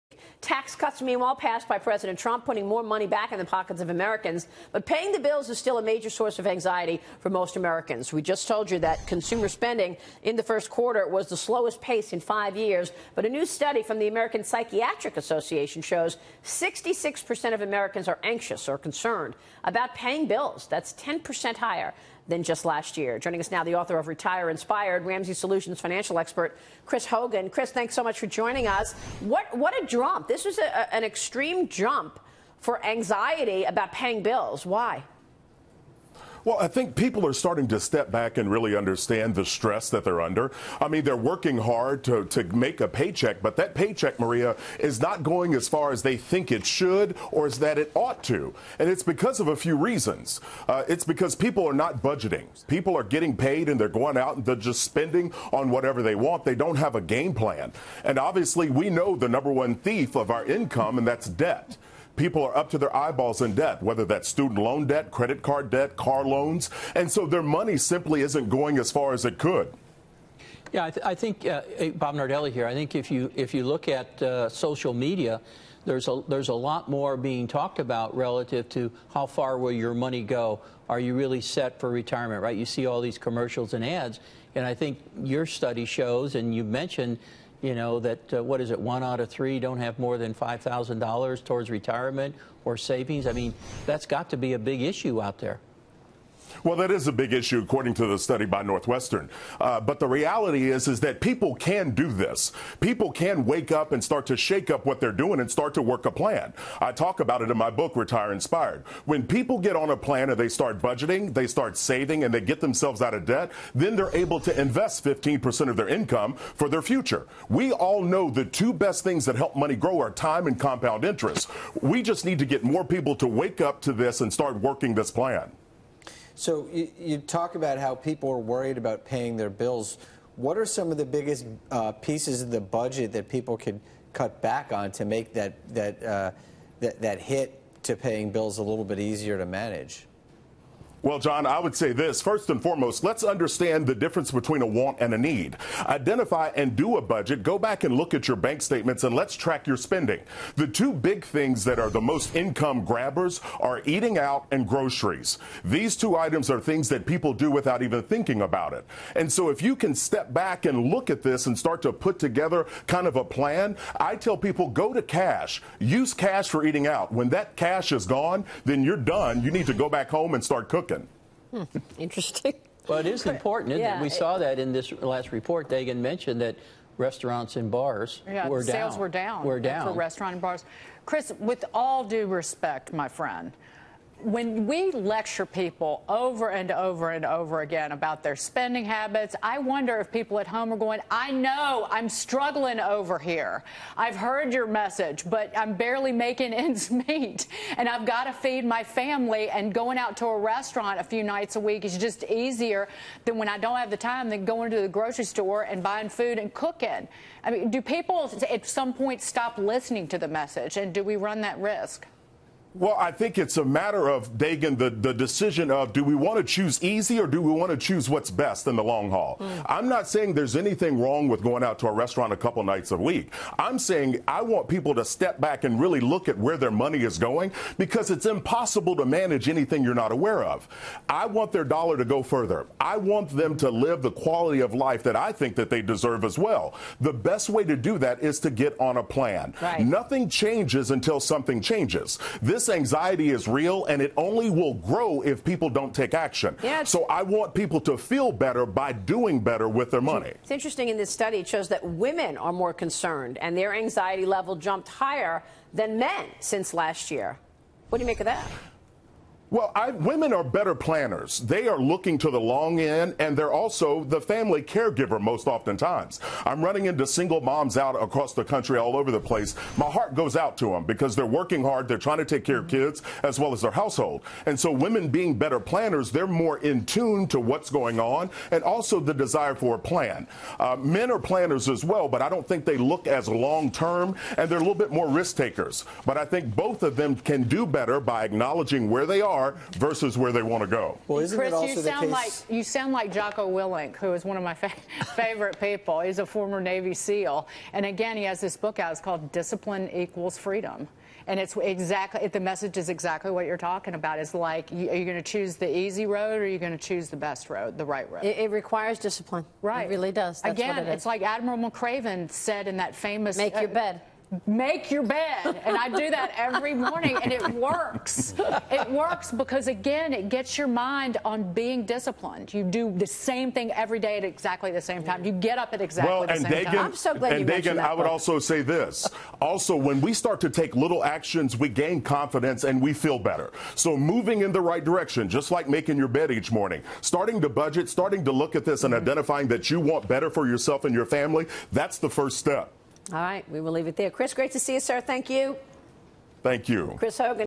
Americans' rising anxiety over paying their bills
"Retire Inspired" author Chris Hogan on a new study about Americans mounting concerns about their ability to pay their bills.